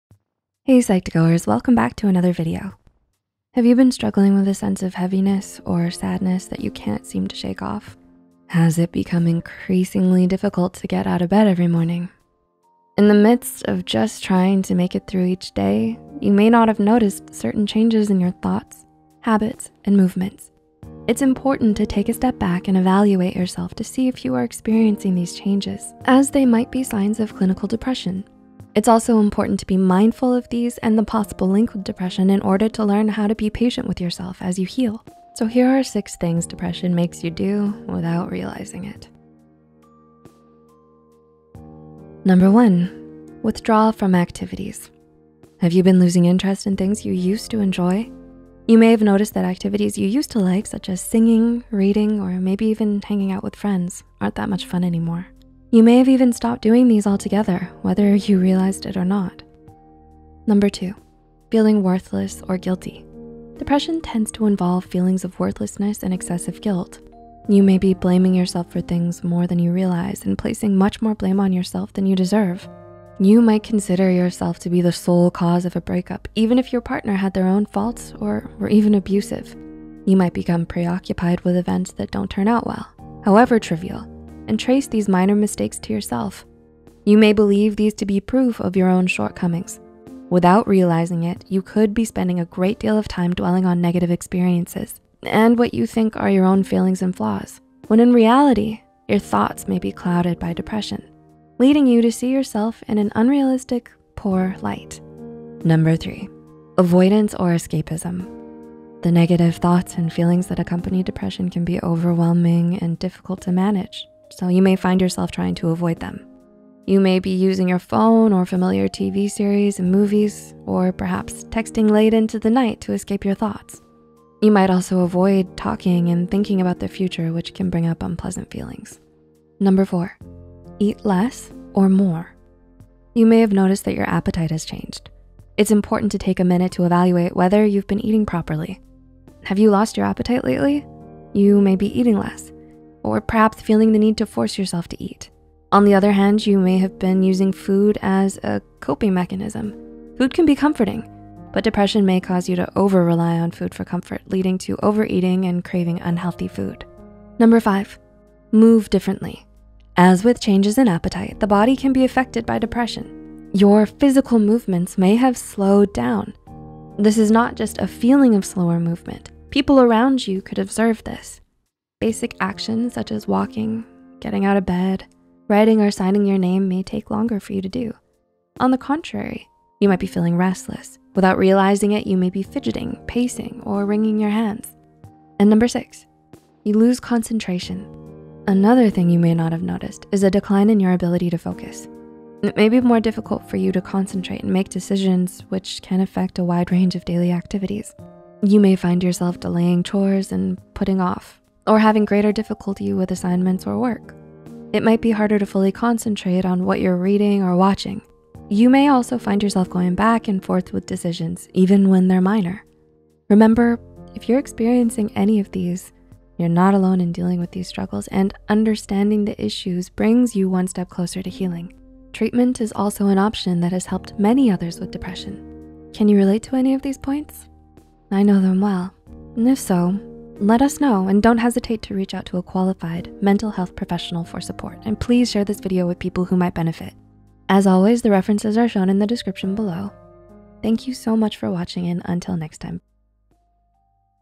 Depression Makes You Do These 6 Things Without Realizing It
Have you been struggling with a deep weight of sadness that you can’t seem to shake off? Has it become increasingly difficult to get out of bed every morning?

It is important to take a step back and evaluate yourself; to see if you are experiencing these changes, as they might be signs of clinical depression.

Writer: Paula C
Script Editor: Caitlin McColl
Script Manager: Kelly Soong
Animator: Faye Miravalles
YouTube Manager: Cindy Cheong

REFERENCES
Diagnostic and Statistical Manual of Mental Disorders: DSM-5. Washington, DC: American Psychiatric Publishing; 2013.​
Medical News Today. (n.d.). What does depression feel like? MediLexicon International.
Schimelpfening, N. (2021, January 29). Symptoms of Clinical Depression. Verywell Mind.
The Depression Project. (2021, October 14). Things You May Not Realize You’re Doing Because of Depression. instagram.com. Retrieved
What Is Depression?